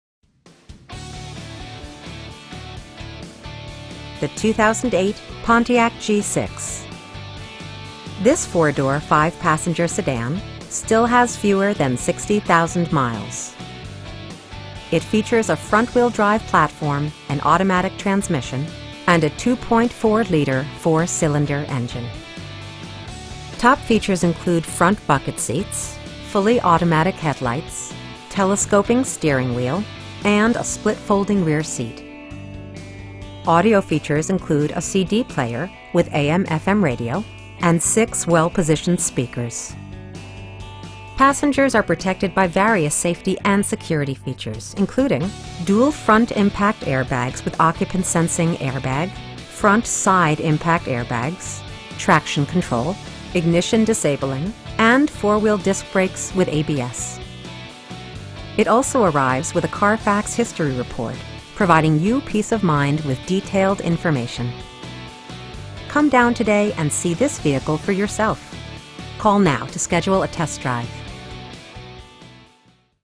2008 Pontiac G6 Value Leader in Marion, IL 62959
Marion Toyota
3300 W Deyoung

The 2008 Pontiac G6. This 4 door, 5 passenger sedan still has fewer than 60,000 miles! It features a front-wheel-drive platform, an automatic transmission, and a 2.4 liter 4 cylinder engine. Top features include front bucket seats, air conditioning, fully automatic headlights, telescoping steering wheel, power door mirrors, power windows, and a split folding rear seat. Audio features include a CD player with AM/FM radio, and 6 well positioned speakers. Passengers are protected by various safety and security features, including: dual front impact airbags with occupant sensing airbag, front SIDE impact airbags, traction control, ignition disabling, and 4 wheel disc brakes with ABS. It also arrives with a Carfax history report, providing you peace of mind with detailed information. Come down today and see this vehicle for yourself.